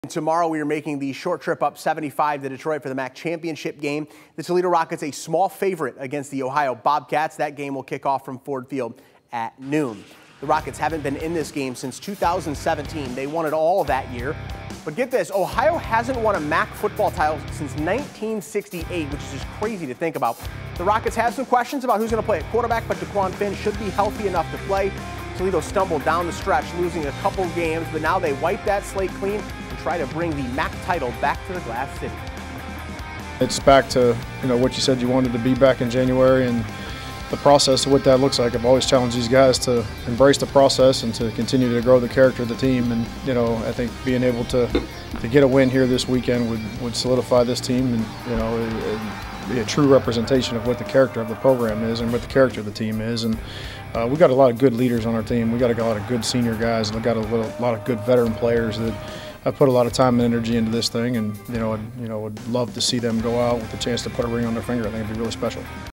University of Toledo vs. Ohio University football MAC Championship game
The Rockets haven't been in this game since 2017. OU hasn't won a MAC football title since 1968.